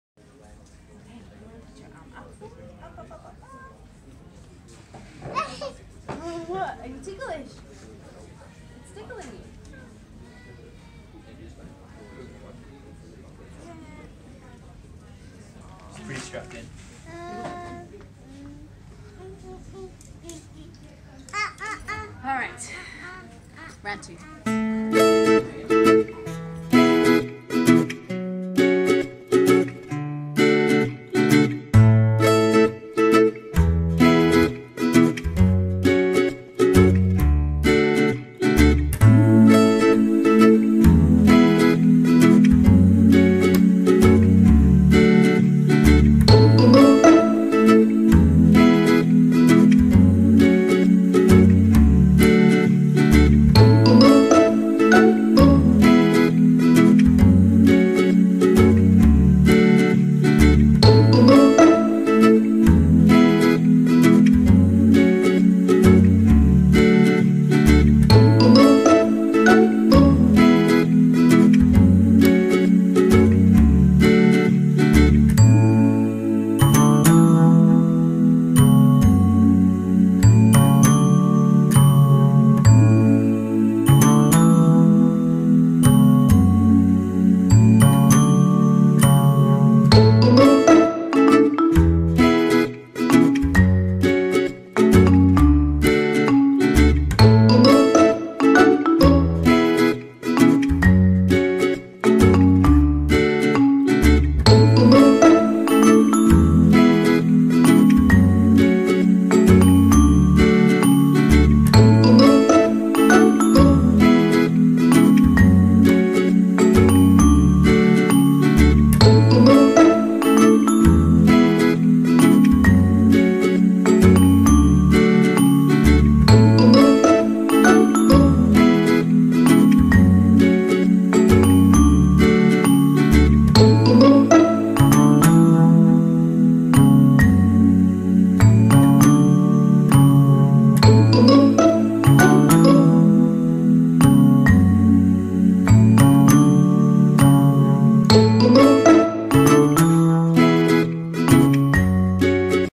LONG ARM CAST
Check out our physio Rash and student AJ apply a long arm cast to our little patient!